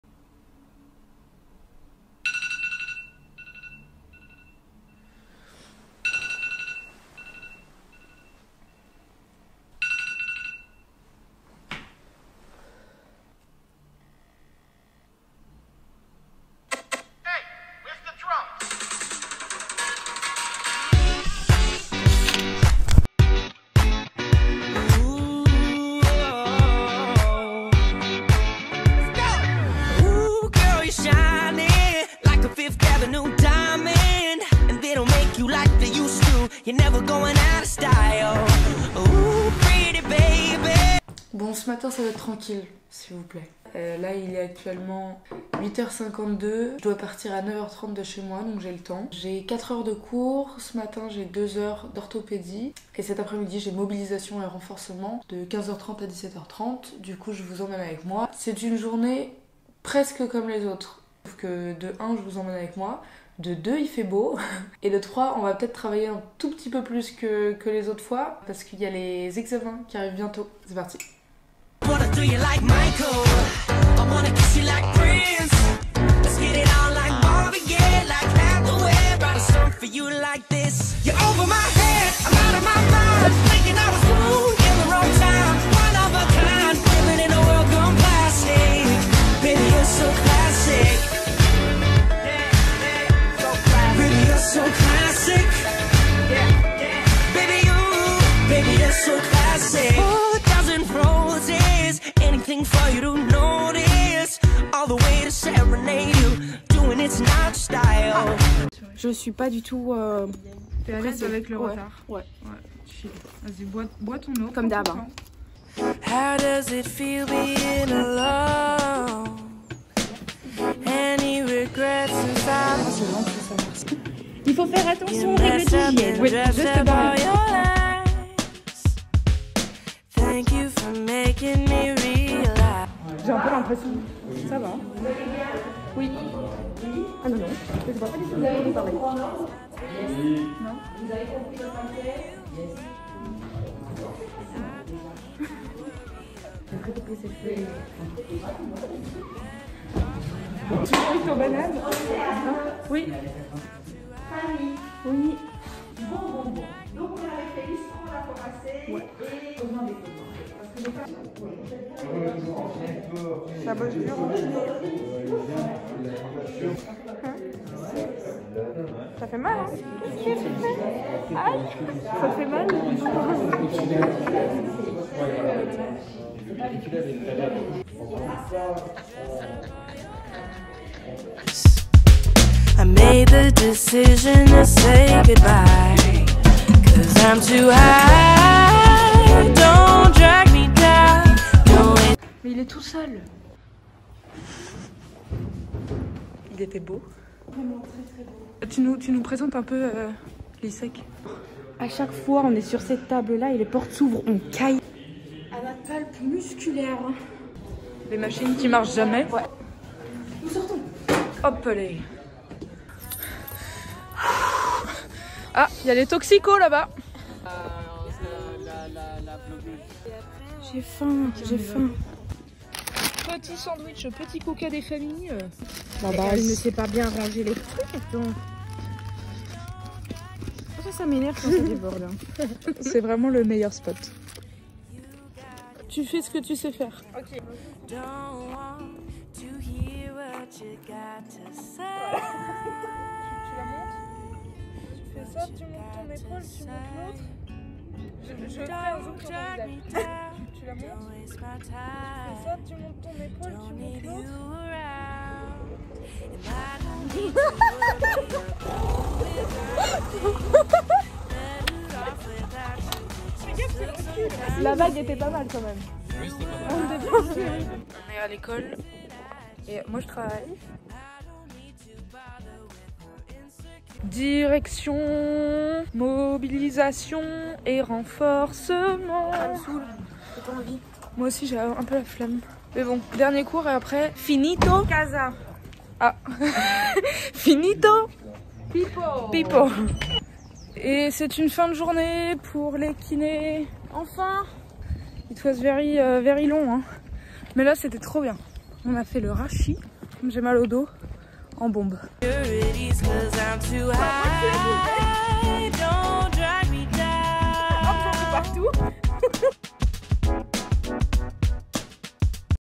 Étudiante en kinésithérapie (une journée presque type)
Salut les copains je suis étudiante en première année de kinésithérapie à l’ISEK à Bruxelles.
J’avais envie de vous partager une journée en présentiel avec moi, j’espère que ça vous plaira !

Matériel :
- iPhone 11
- iPad 11 pro
- Lumix g 80
- LumaFusion

Réseaux :
Instagram : O C É A N E V I D I L (@oceanevidil) • Photos et vidéos Instagram